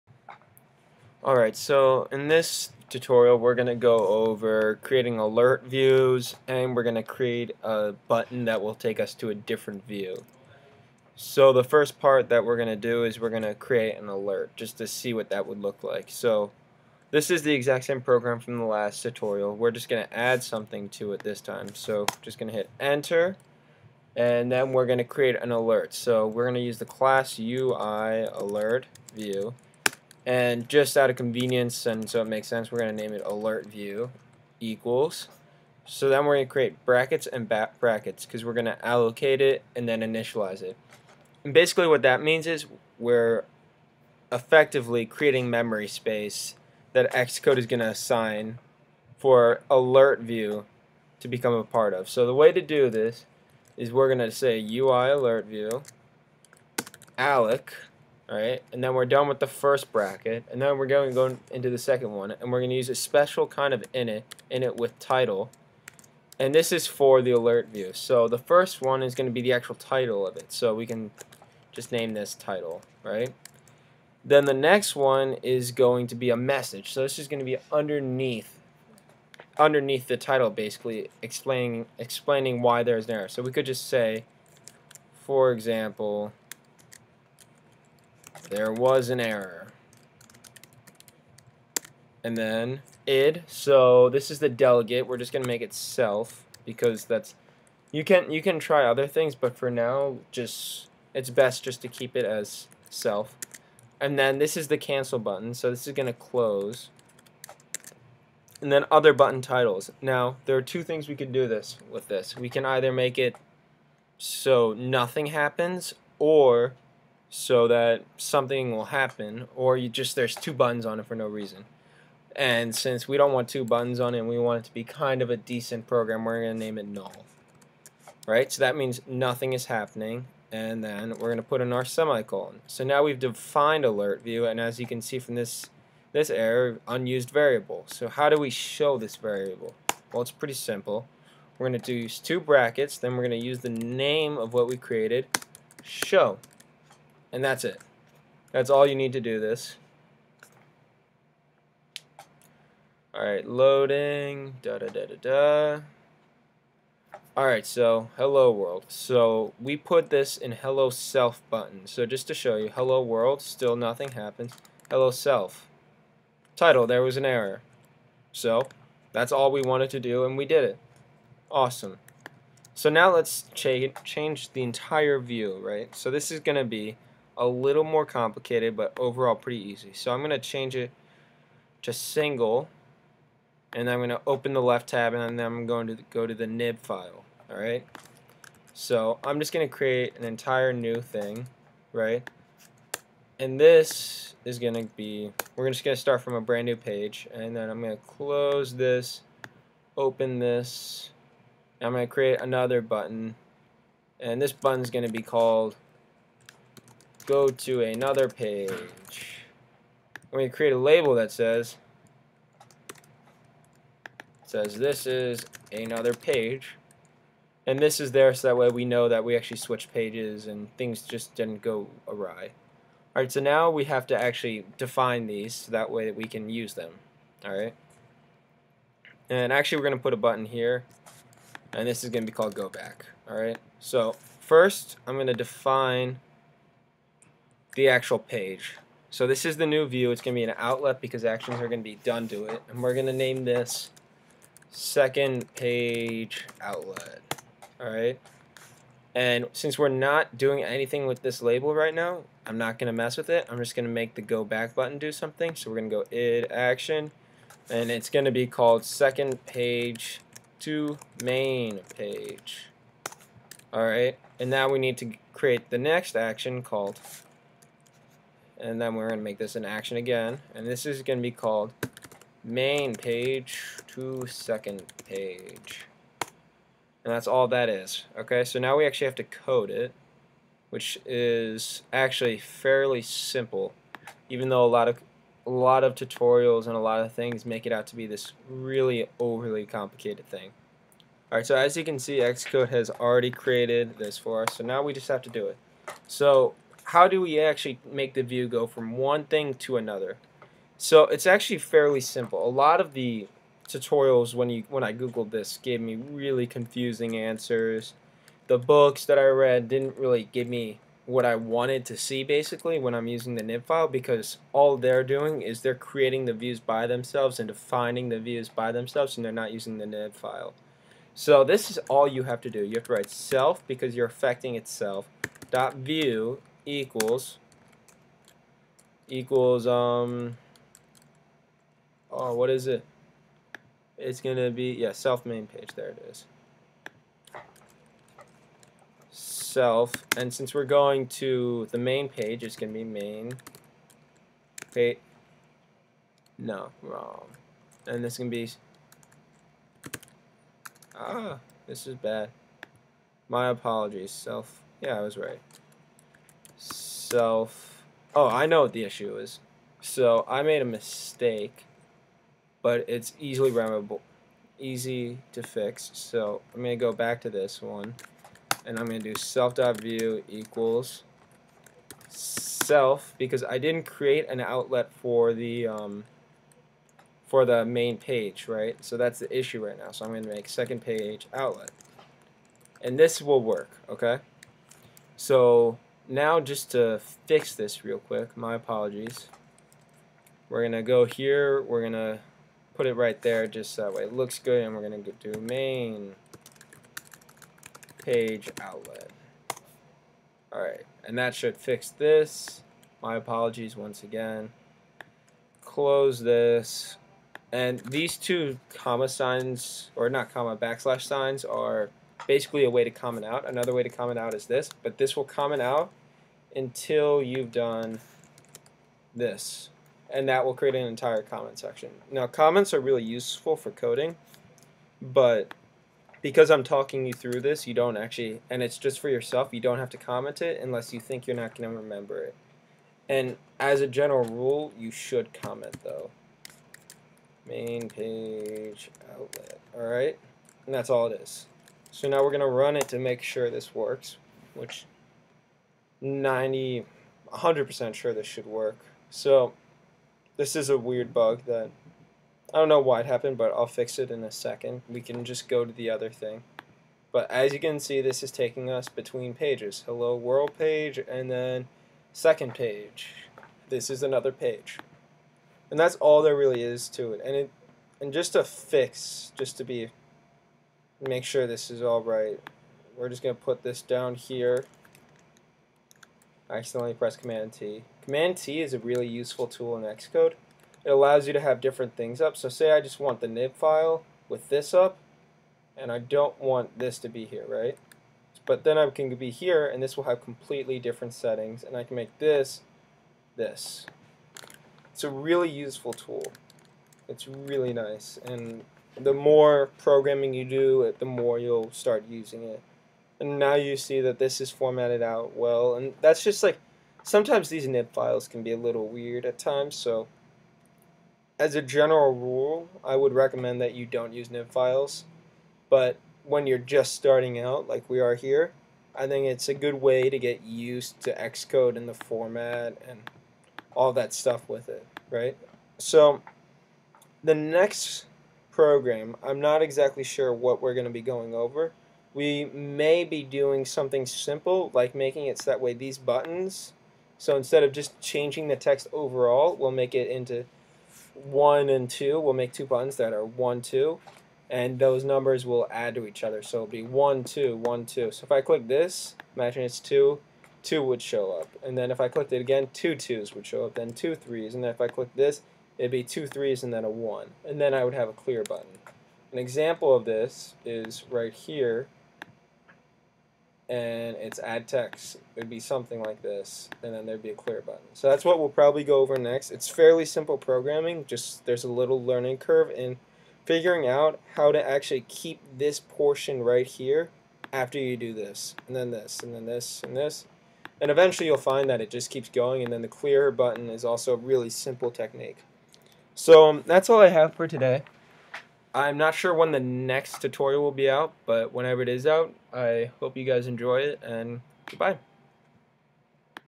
UIAlertView and Changing views in IOS
this goes over the basics of programming with alert and what to do with them and how to show them. It does not show how to make use of the input from the buttons but that will be covered later on. Furthermore it shows how to create different views within an app through a fairly simple method